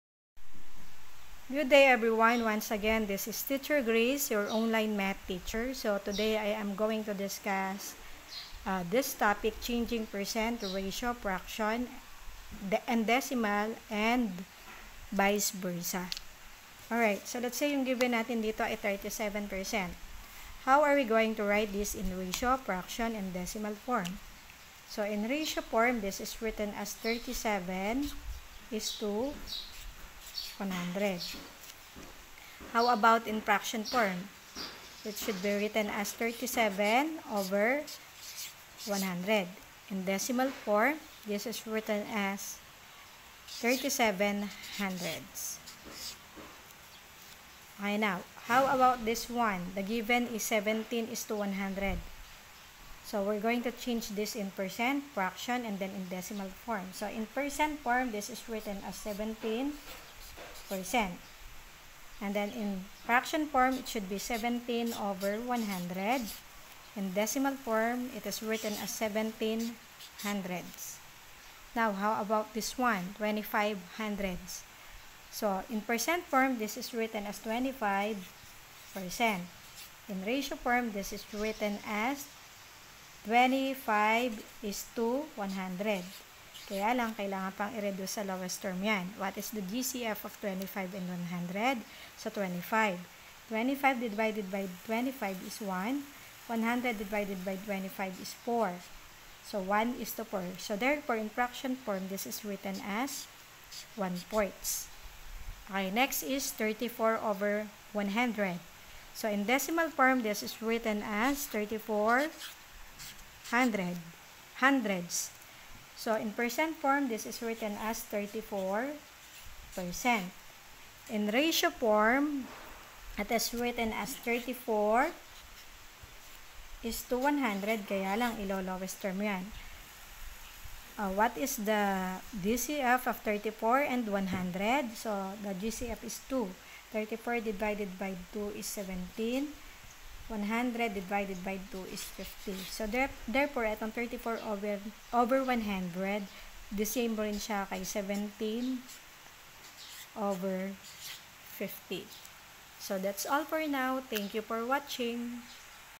Changing Percent to Ratio, Fraction, Decimal and vise versa
This video is created to help Grade 5 pupils to understand the said topic.